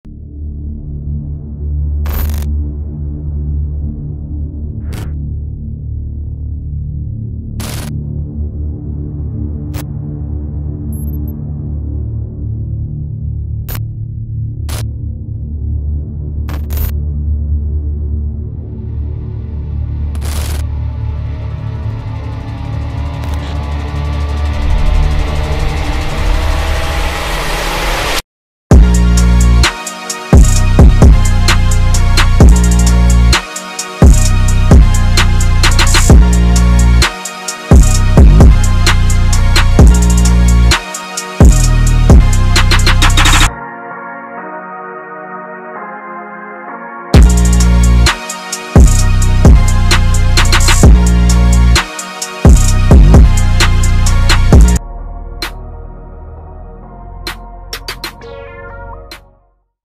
Black Howl • 10.5.17
The NC State Football team will don these special Adidas uniforms against Louisville on Thursday night, October 5.